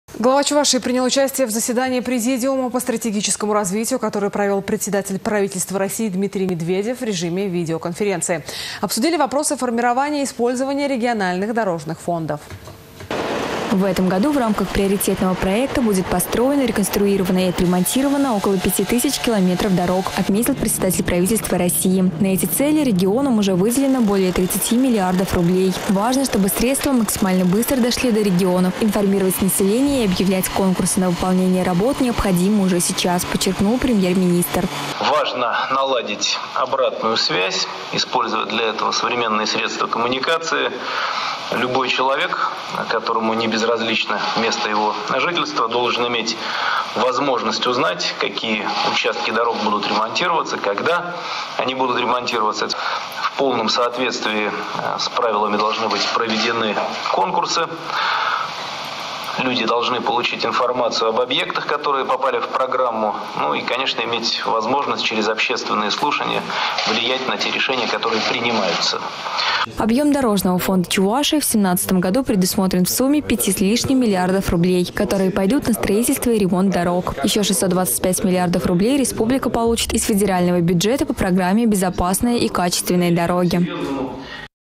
Дмитрий Медведев провел заседание президиума по стратегическому развитию
Глава Чувашии принял участие в заседании президиума по стратегическому развитию, которое провел председатель правительства России Дмитрий Медведев в режиме видеоконференции. Обсудили вопросы формирования и использования региональных дорожных фондов.